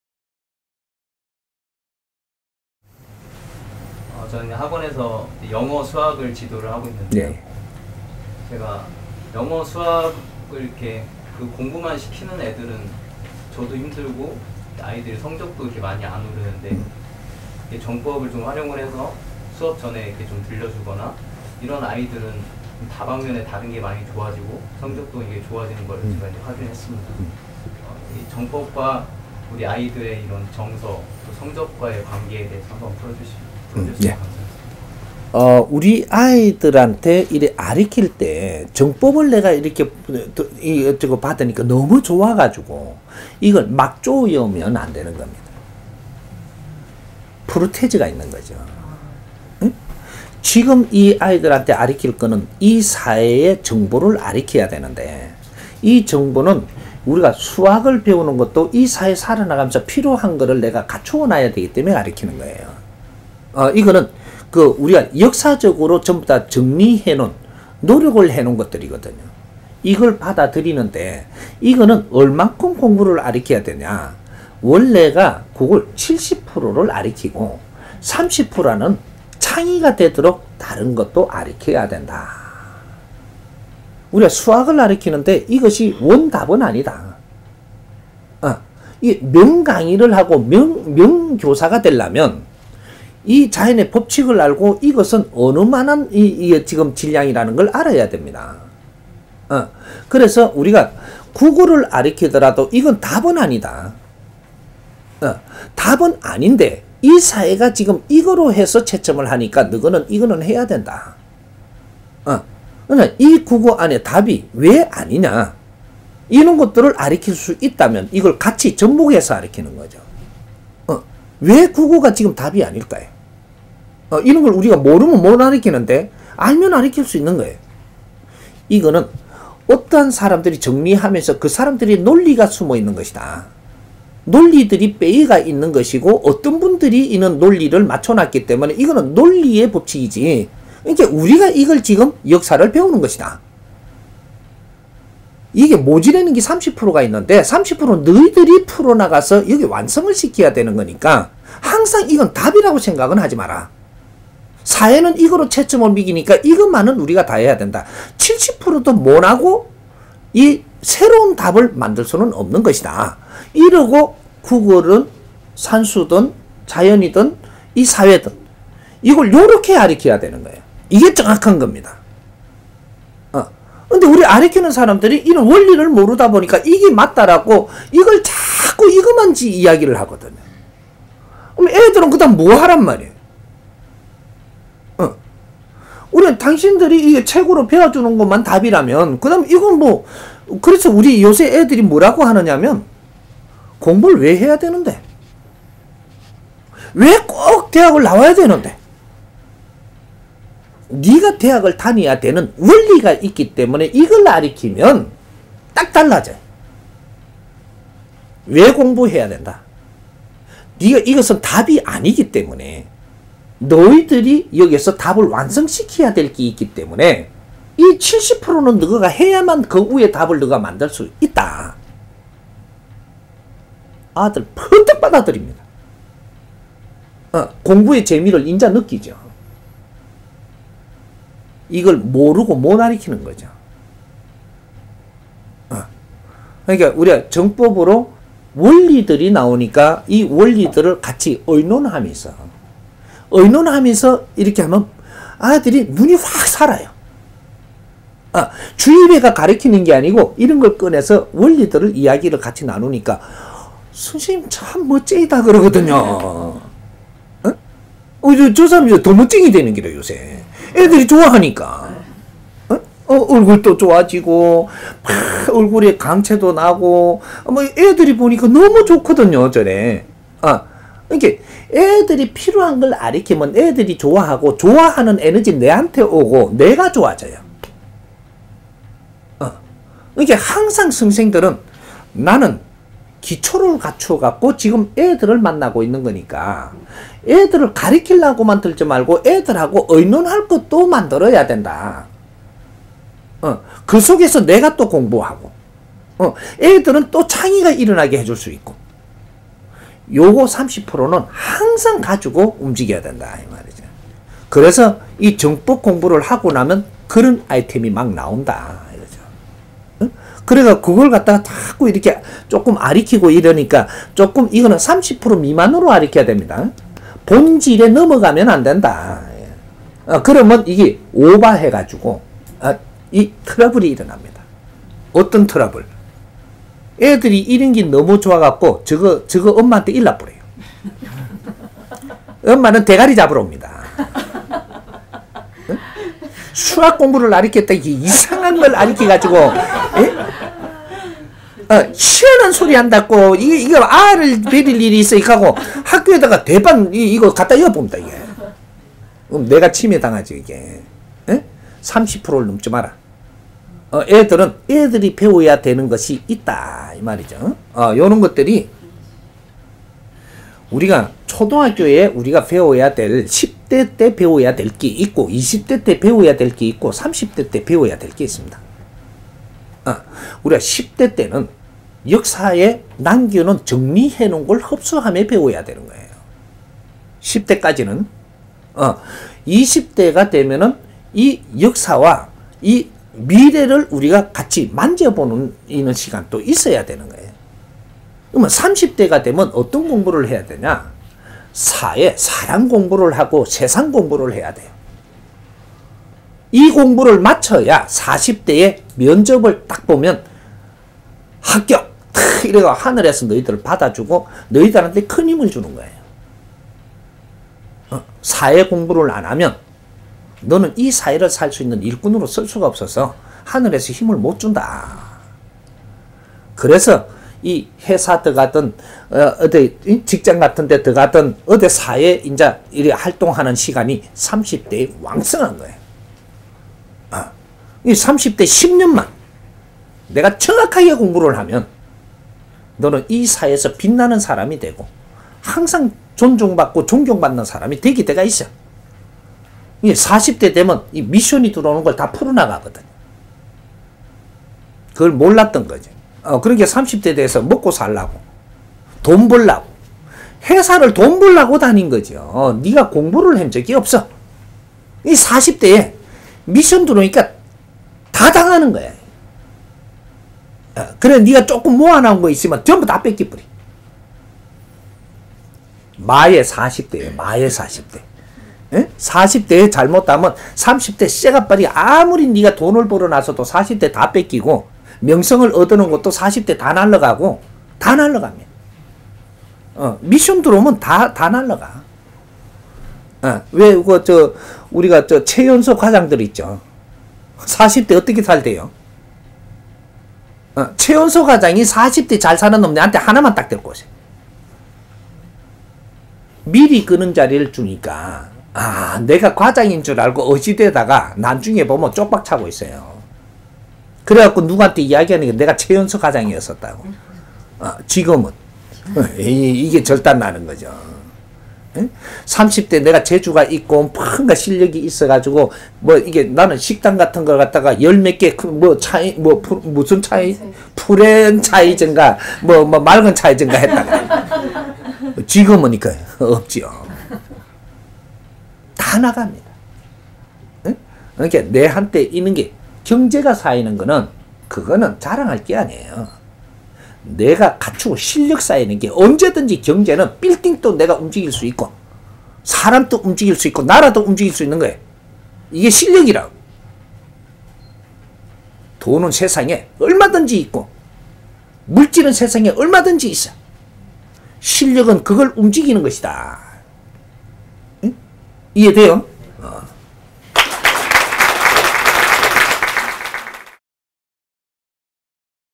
[정법강의] 4753강 정법이 아이들에게 미치는 영향
질문 : 저는 학원에서 영어, 수학을 지도하고 있는데요. 제가 영어, 수학을 공부만 시키는 애들은 좀 더 힘들고, 성적도 많이 안 오르는데, 정법을 조금 활용을 해서 수업 전에 들려주거나, 이런 아이들은 다방면에 다른 것이 많이 좋아지고 성적도 좋아지는 것을 제가 확인을 했습니다. 정법과 이런 우리 아이들의 정서, 또 성적과의 관계에 대해서 한 번 풀어주셨으면 감사하겠습니다.

강의 일자 : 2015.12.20 전주 / 정법시대 : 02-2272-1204